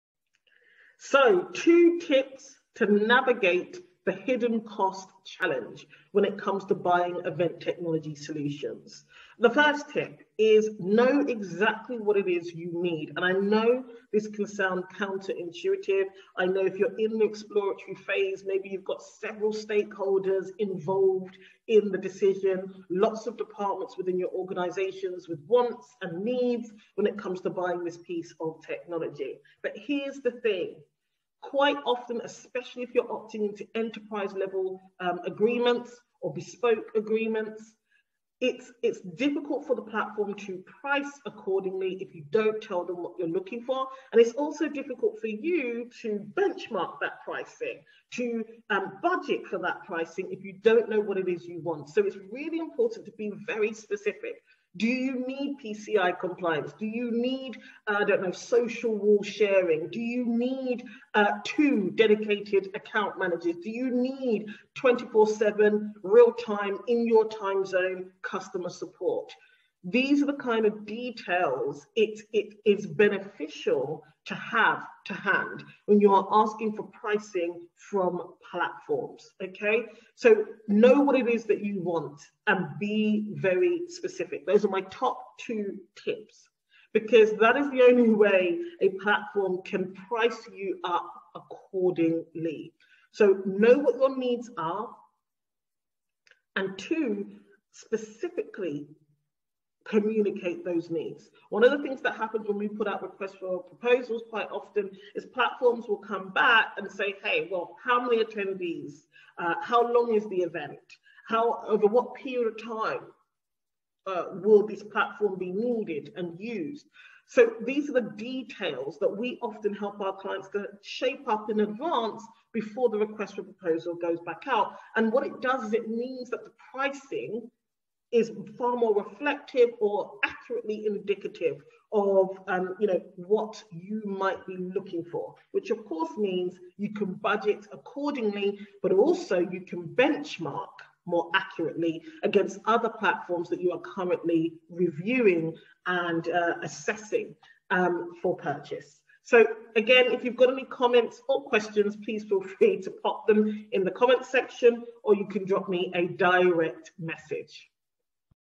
2 Tips For Navigating Event Tech Pricing │ FAQ's about Event Technology
In this series EventMind's CEO Ashanti will be spilling all tips and tricks ensuring you're getting the best out of event technology, this video gives you 2 tips for when navigating event technology pricing.